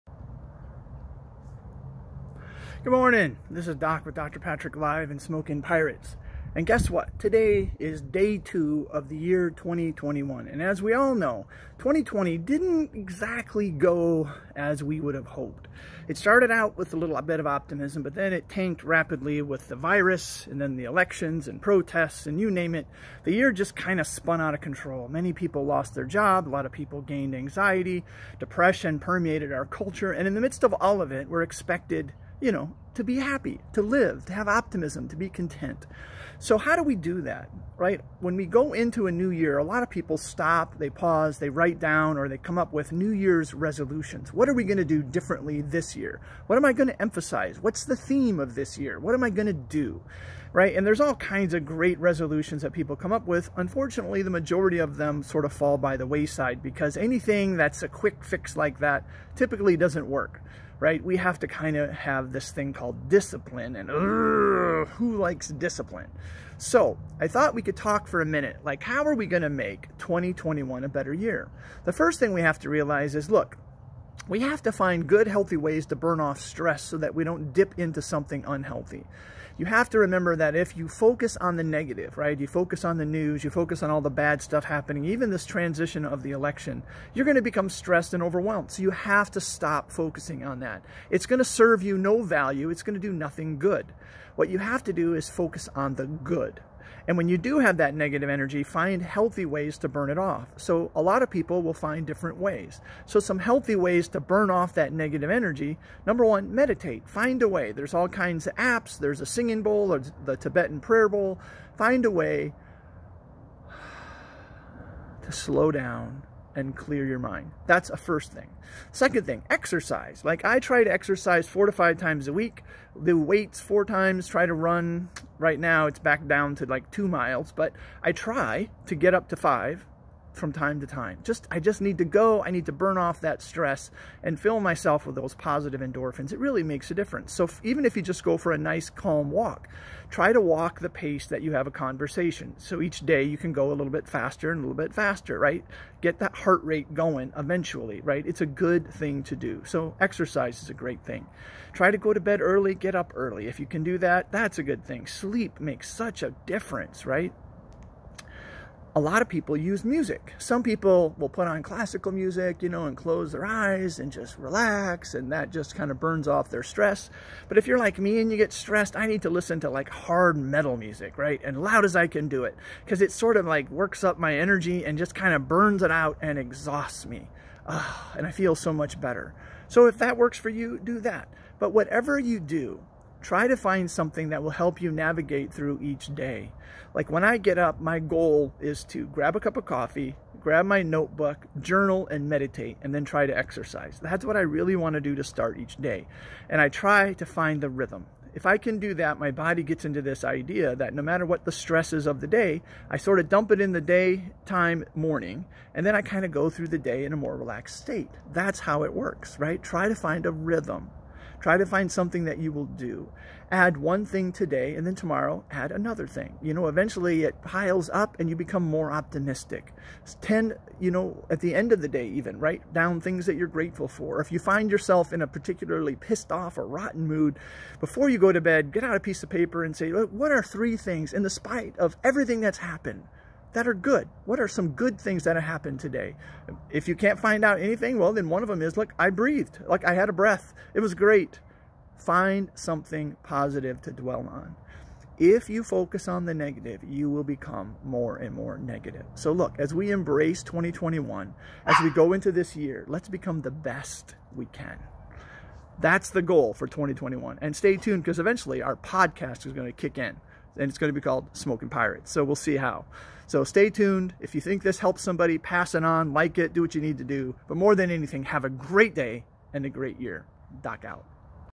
How to embrace 2021 with optimism
As we break into another year let's grab 2021 and squeeze all the optimism and hope out of it. We can choose to overcome, to be positive and to truly experience peace. Here is how!